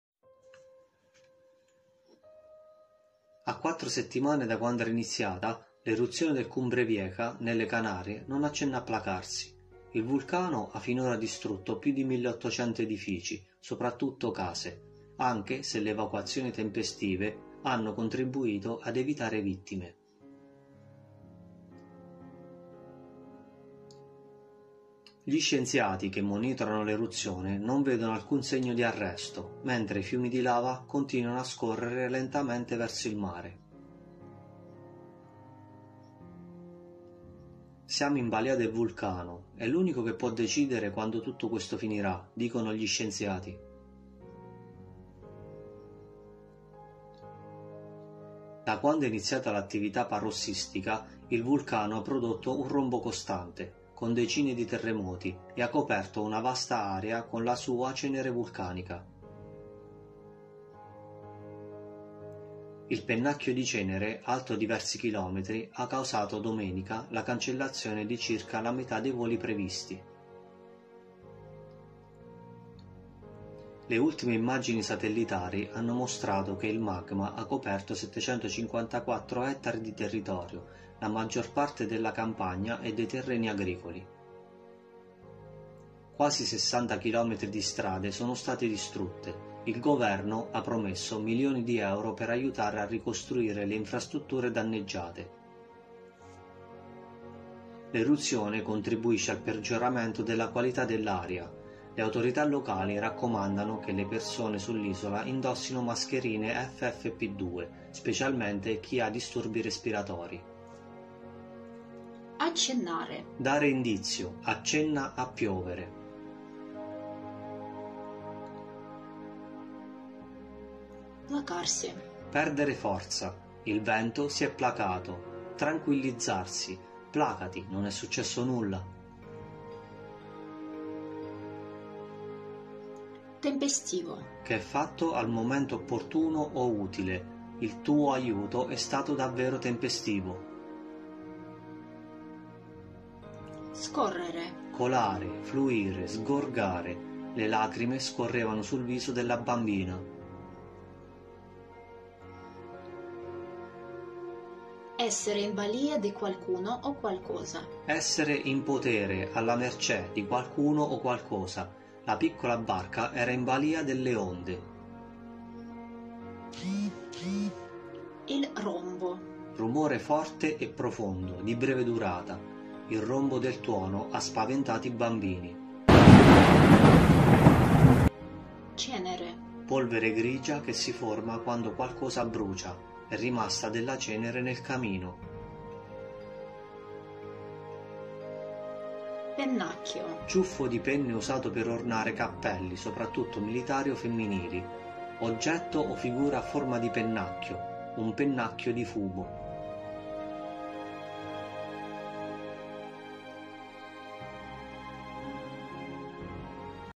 Il Cumbre Vieja
This video is about the eruption of the Cumbre Vieja volcano on the Spanish island of La Palma